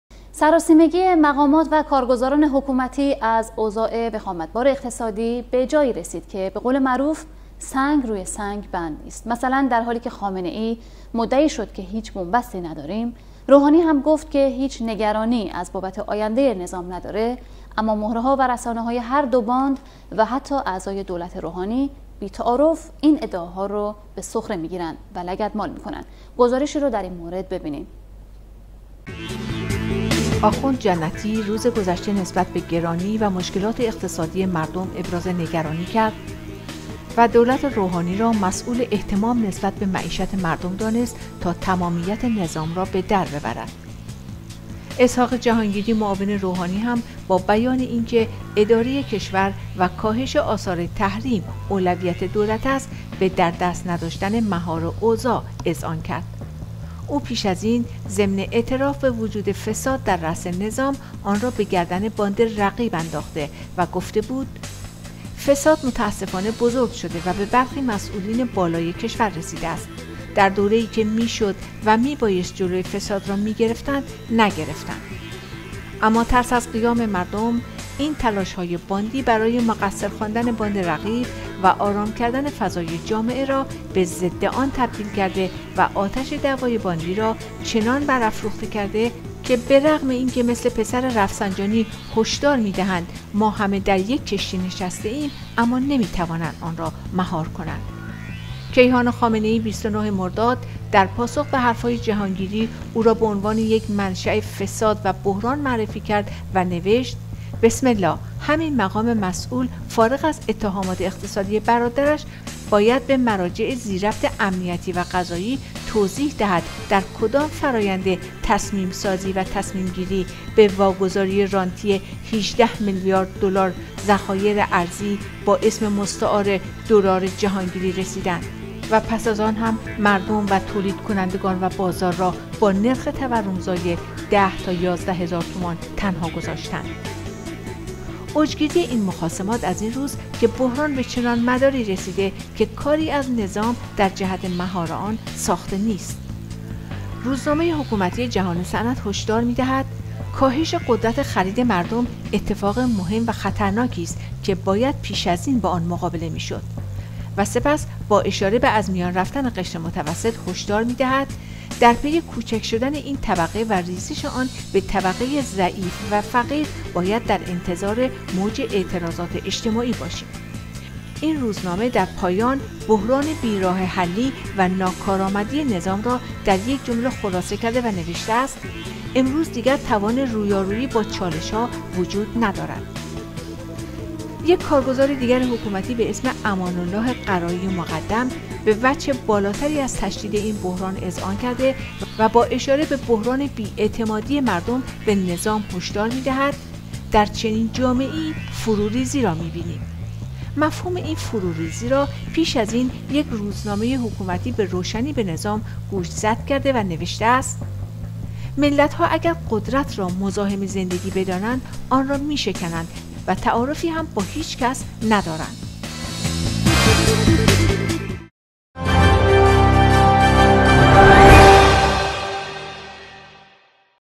انتظار امواجه اعتراضات و قیام، پیامدِ فساد دزدی گرانی افسار گسیخته
سیمای آزادی تلویزیون ملی ایران ۳۰ مرداد۹۷